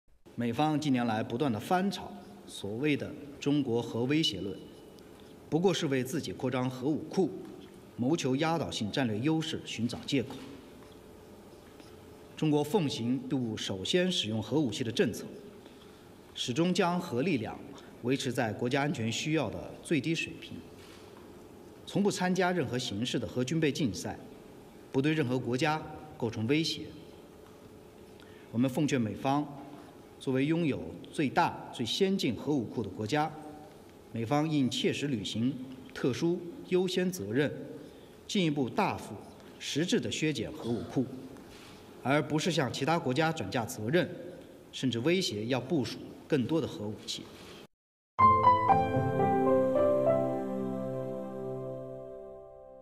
美官员指责中国快速扩张核武库 中方回应
中国外交部发言人林剑6月11日主持例行记者会。

有记者提问：据报道，6月7日，美国白宫国家安全委员会防扩散事务高级主任普拉奈·瓦迪在美军控协会年度论坛上发表讲话，指责中国快速扩张核武库，声称为应对中俄等对手挑战，美将采取更具竞争性的核战略，在未来几年可能需部署更多核武器。中方对此有何评论？

林剑：中方反对美方有关言论。美方近年来不断翻炒所谓的“中国核威胁”论，不过是为自己扩张核武库、谋求压倒性战略优势寻找借口。

中国奉行不首先使用核武器政策，始终将核力量维持在国家安全需要的最低水平，从不参加任何形式的核军备竞赛，不对任何国家构成威胁。

我们奉劝美方，作为拥有最大、最先进核武库的国家，美方应切实履行特殊、优先责任，进一步大幅、实质削减核武库，而不是向其他国家转嫁责任，甚至威胁要部署更多核武器。